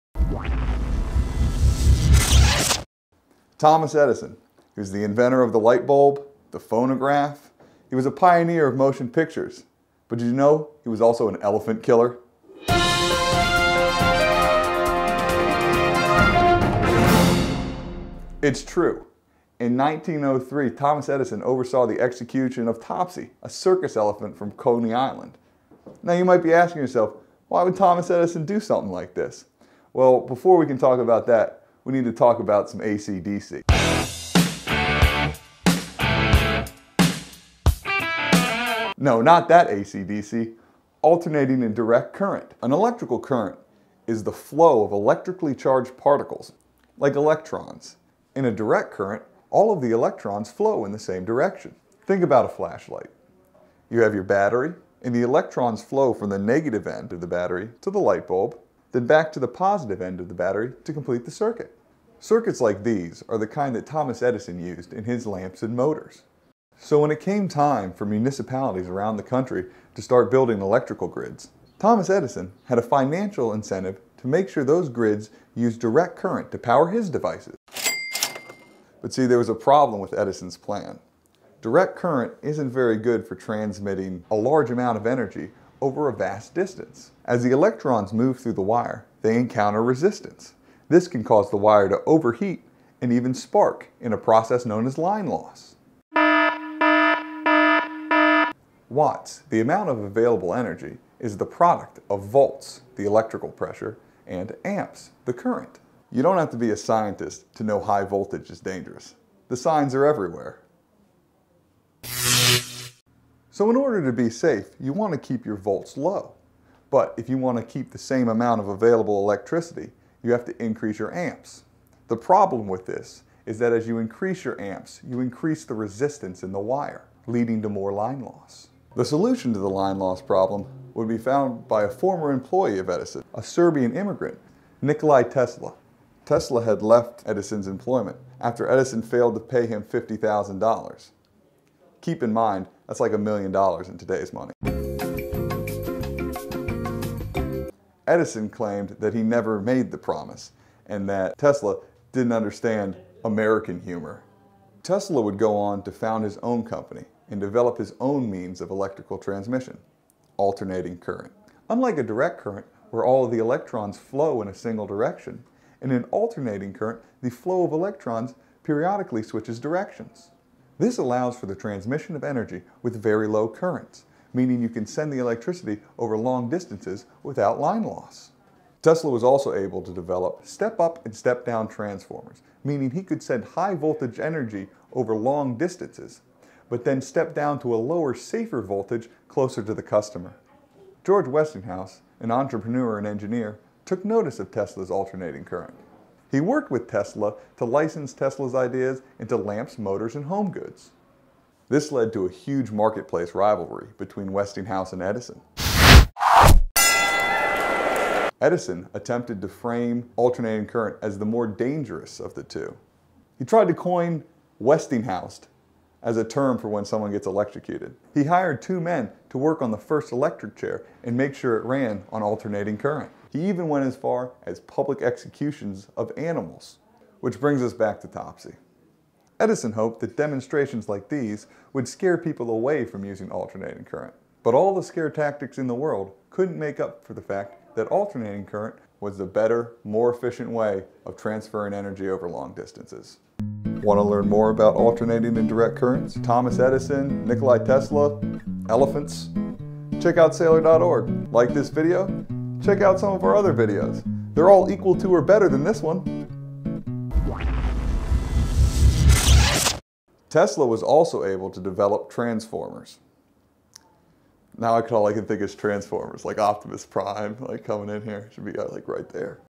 Thomas Edison's Electrifying Secret
AC/DC, Thomas Edison, an Elephant and how they all connect.